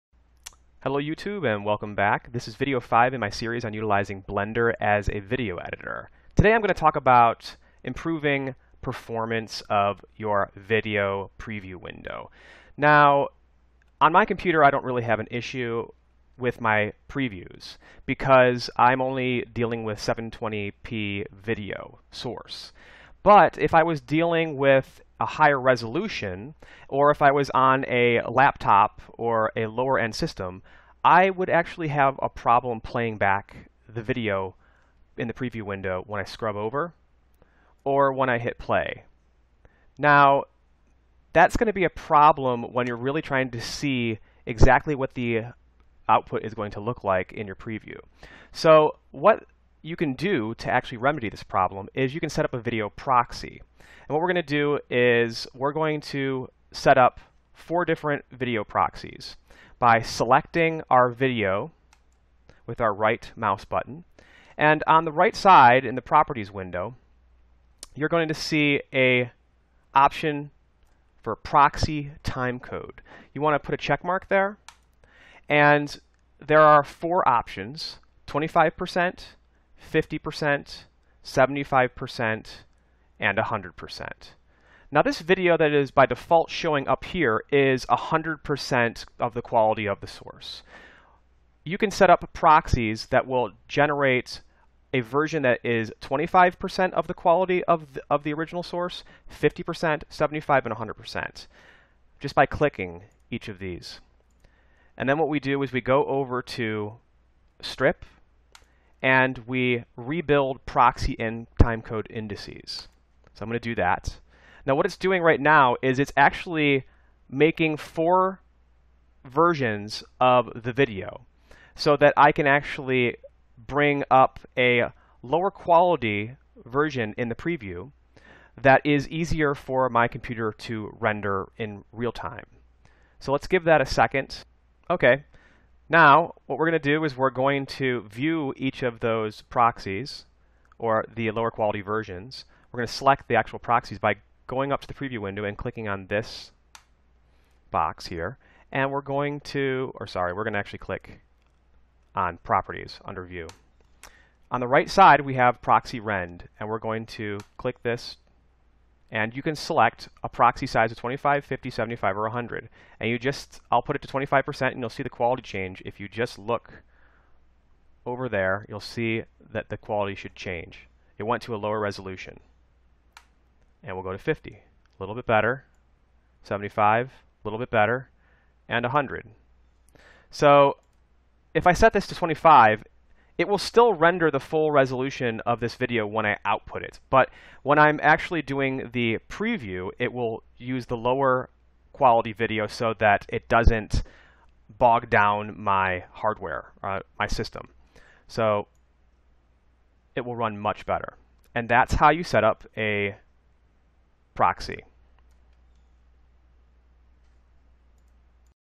Blender Video Editing - Part 5 (Improve Video Preview Performance)
Learn to Video Edit with Blender

All My Latest Content in 1 place

This video will show you how to setup a video Proxy so that you can improve the performance of video in your preview window when editing high resolution footage.